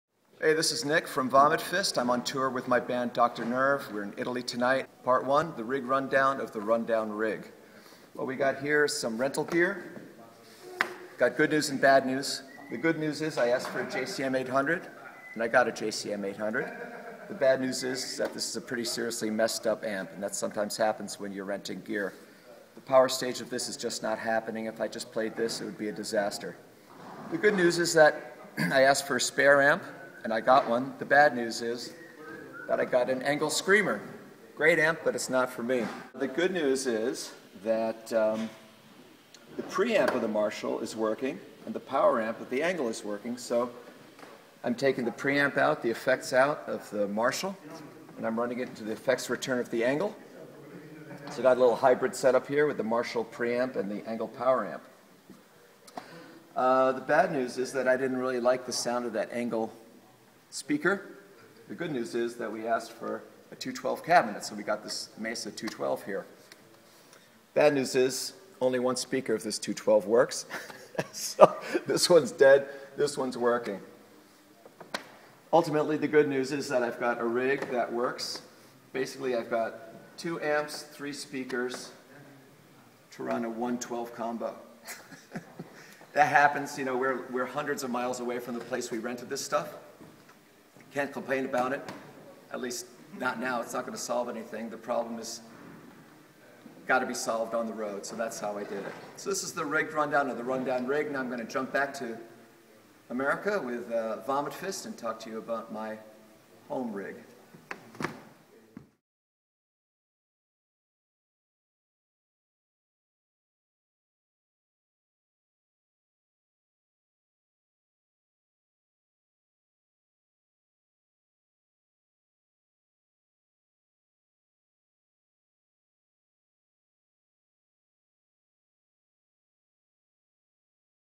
When guitar gear fails on the road, Doctor Nerve / Vomit Fist
Nick shows how he patched together a guitar rig using selectively failing rental gear during the 2014 Doctor Nerve tour of Europe.
Make sure you check out the cute cat at the end.